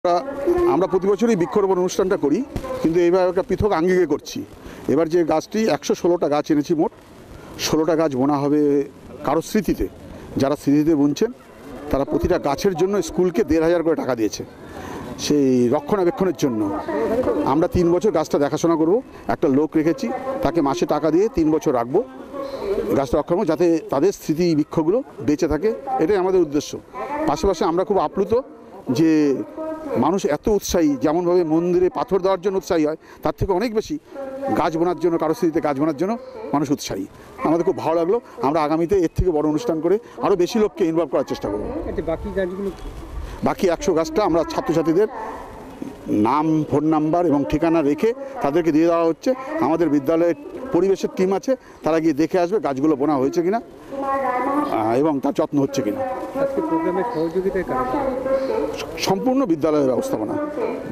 SILIGURI BIKHOROPON/প্রিয়জনের স্মৃতির উদ্দেশ্যে বৃক্ষরোপণ হল হায়দরপাড়া স্কুলে
প্রিয়জনের স্মৃতির উদ্দেশ্যে বৃক্ষরোপণ হল হায়দরপাড়া স্কুলে

শিলিগুড়ি, ২৬ জুলাই: প্রতিবছরই বনমহোৎসব পালন করে হায়দরপাড়া বুদ্ধ ভারতী উচ্চ বিদ্যালয় (উঃ মাঃ)। তবে এবছর দিনটি পালন হল তবে তা একটু অন্য আঙ্গিকে। শহরের কিছু শুভবুদ্ধিসম্পন্ন ও সমাজ সচেতন মানুষ তাঁদের প্রিয়জনের স্মৃতির উদ্দেশ্যে ১৬টি বৃক্ষরোপণ করলেন। শুধু বৃক্ষরোপণই নয়, স্কুলকে গাছের রক্ষণাবেক্ষণের জন্য দেড় হাজার টাকা করে তুলেও দিলেন।
শুক্রবার দুপুরে হায়দরপাড়া বুদ্ধ ভারতী উচ্চ বিদ্যালয়ের খেলার ময়দানে বৃক্ষরোপণ অনুষ্ঠানটি করা হয়। স্কুলের ছাত্র–ছাত্রীদের নিয়ে বর্নাঢ্য শোভাযাত্রা বের করা হয়। হয় সাংস্কৃতিক অনুষ্ঠানও। বৃক্ষরোপণ কর্মসূচীতে উপস্থিত ছিলেন ভক্তিনগর থানার আইসি সুজয় তুঙ্গা, পশ্চিমবঙ্গ বিজ্ঞান মঞ্চের কার্যকরি সভাপতি গোপাল দে, ন্যাফের সহ সম্পাদক প্রদীপ নাগ, জ্যোৎস্না আগরওয়াল–সহ শিক্ষক–শিক্ষিকারা। স্কুলের প্রধান শিক্ষক স্বপনেন্দু নন্দী বলেন, ‘মোট ১১৬টি গাছ লাগানো হবে। এদিন ১৬টি গাছ শহরের কিছু শুভবুদ্ধিসম্পন্ন ও সমাজ সচেতন মানুষ তাঁদের প্রিয়জনের স্মৃতির উদ্দেশ্যে নিজের হাতে ১৬টি গাছ পুঁতলেন। রক্ষণাবেক্ষণের জন্য দিলেন দেড় হাজার টাকা। যে টাকা দিয়ে আমরা এখানে একজনকে নিয়োগ করব গাছগুলি পরিচর্যা করার। বাকি গাছগুলি ছাত্রছাত্রীদের দেওয়া হবে। বৃক্ষরোপণকে ঘিরে এখন সবাই বেশ উৎসাহী। বনমহোৎসব পালনের সময় পার হয়ে গেলেও, সারাবছরই দিনটি পালন করা যায়। আমরা তাই একটু দেরি করেই দিনটি পালন করলাম। আগামীবছর বৃক্ষরোপণ কর্মসূচী আরও বৃহৎ আকারে করব।’